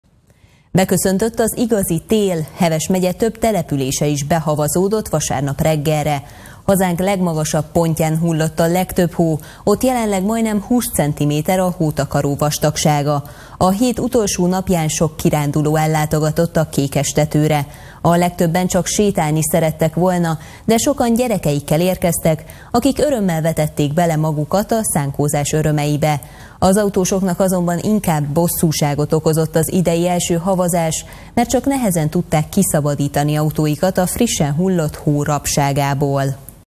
Beköszöntött az igazi tél - 2022.12.12.
Beköszöntött az igazi tél, Heves megye több települése is behavazódott vasárnap reggelre. Hazánk legmagasabb pontján hullott a legtöbb hó, ott jelenleg majdnem 20 centiméter a hótakaró vastagsága. Sok kiránduló ellátogatott a Kékestetőre a hét utolsó napján. A legtöbben csak sétálni szerettek volna, de sokan gyerekeikkel érkeztek, akik örömmel vetették bele magukat a szánkózás örömeibe. Az autósoknak azonban inkább bosszúságot okozott az idei első havazás, mert csak nehezen tudták kiszabadítani autóikat a frissen hullott hó rabságából.